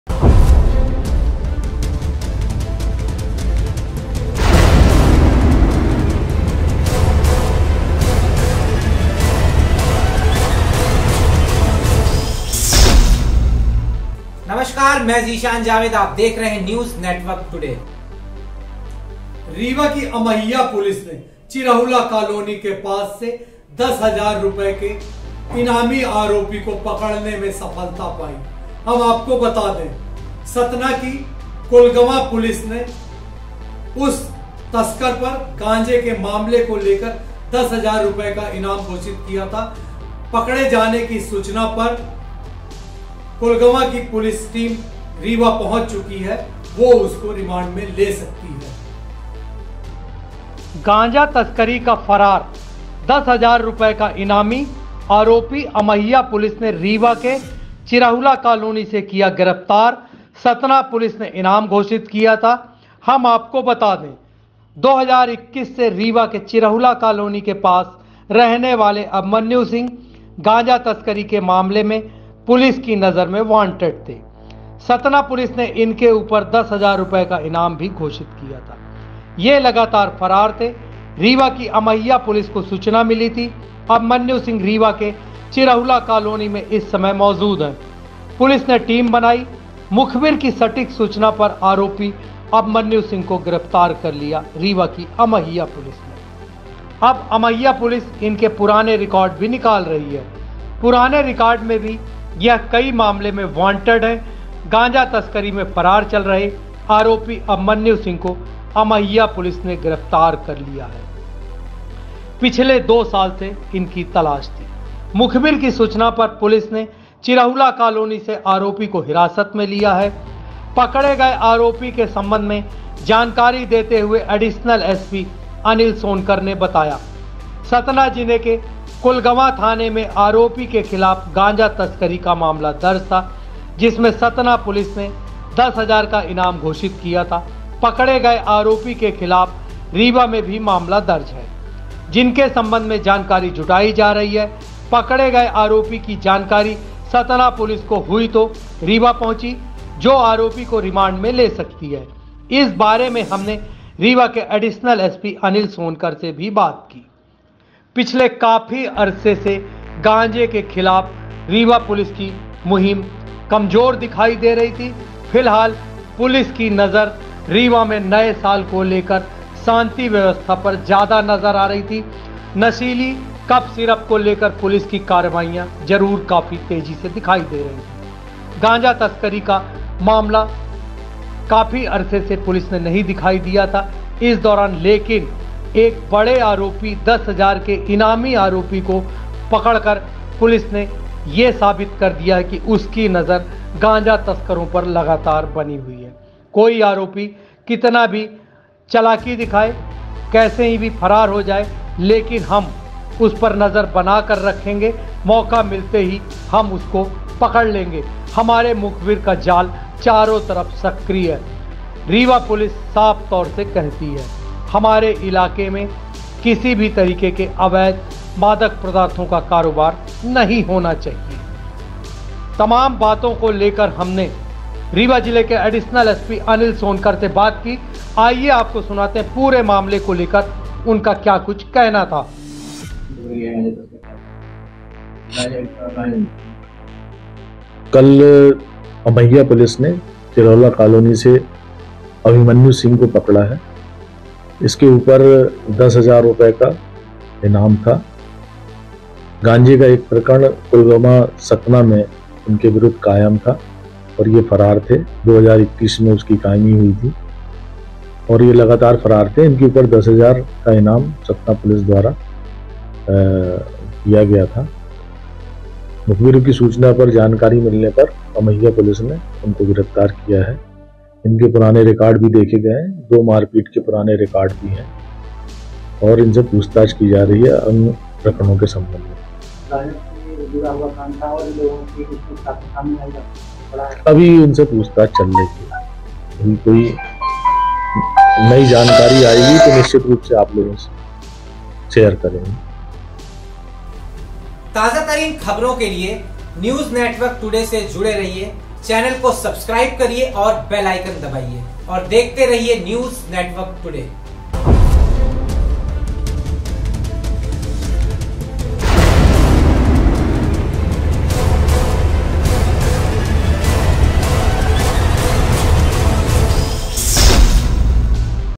गांजा तस्करी का फरार ₹10000 का इनामी अमहिया पुलिस ने चिराहुला कॉलोनी से किया गिरफ्तार ||NNT
गांजा तस्करी का फरार ₹10000 का इनामी अमहिया पुलिस ने चिराहुला कॉलोनी से किया गिरफ्तार सतना पुलिस ने घोषित किया था इनाम